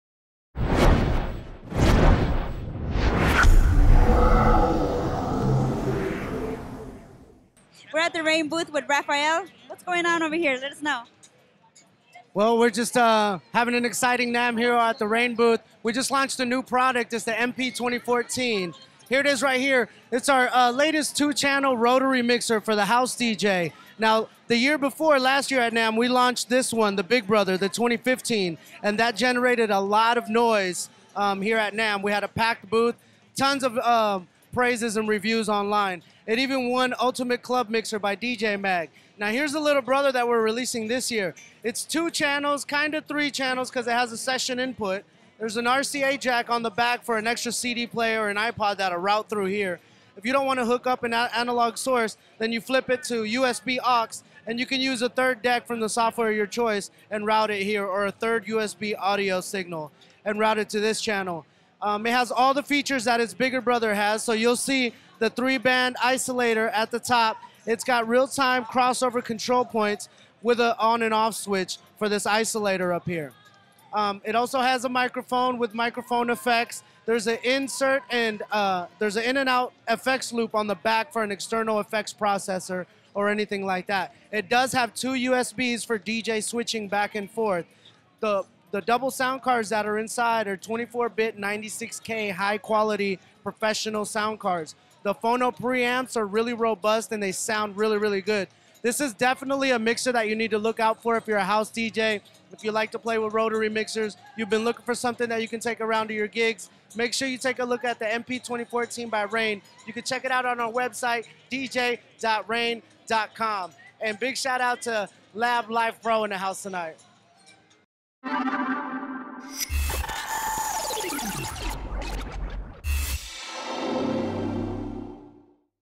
LabLifePro @NAMM 2016 with Rane: Introducing MP2014
Lucy Sarabia and DJ Icewater got to check the new Rane MP2014 Rotary Mixer. This is the little brother to the 2015 Rane mixer introduced last year at NAMM 2015. Check here as Rafael from the Rane booth at NAMM does a run trough some of the functions exclusive for LabLifePro.